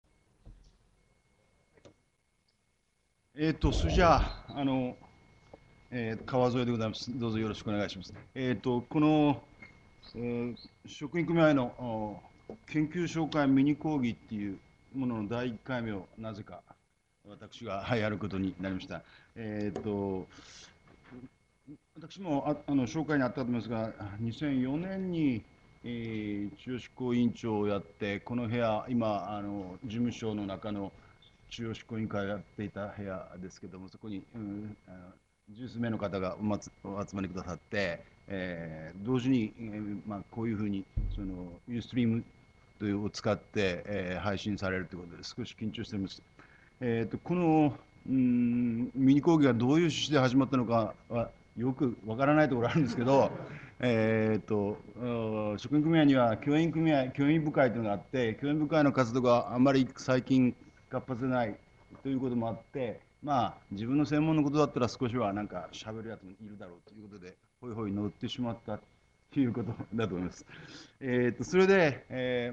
大学は「組合」としてはじまった(前編)1/3
講師：川添信介　1955年佐賀県生まれ。京都大学文学研究科教授。大阪市立大学を経て、1996年から文学研究科で西洋哲学史専修で、中世哲学史を担当。トマス・アクィナスを中心としたスコラ哲学が専門。著書に『水とワイン西欧13世紀における哲学の諸概念』（京都大学学術出版会）2005年など。
また、2004年度に京都大学職員組合中央執行委員長を務めた。
※今回の録画では、音声の乱れ、録画中断が生じてしまいました。悪しからずご了承下さい。